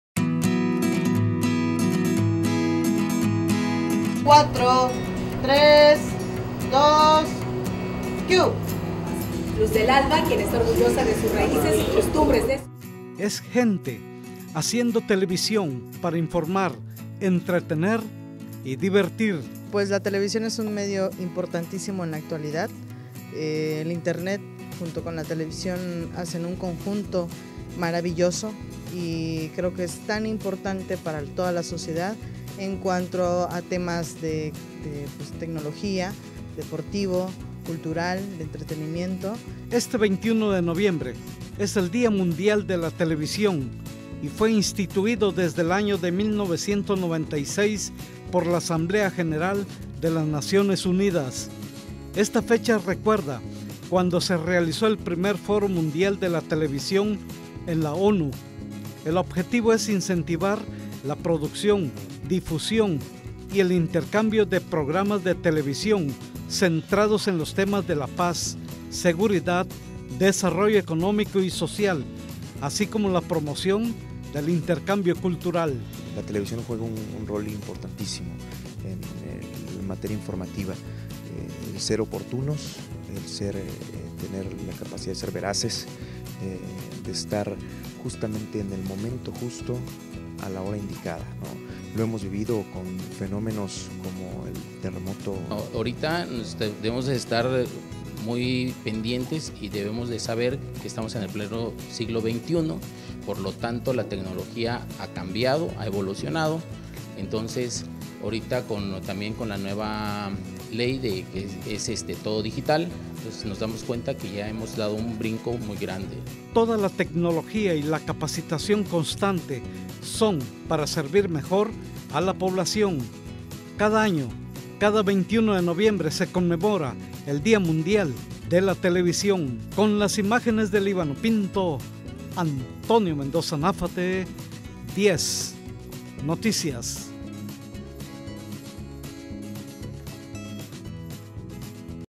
21 de Noviembre Día Mundial de la Televisión
Es gente haciendo televisión para informar, entretener y divertir.
Este 21 de noviembre, es el Día Mundial de la Televisión, y fue instituido desde el año de 1996 por la Asamblea General de las Naciones Unidas.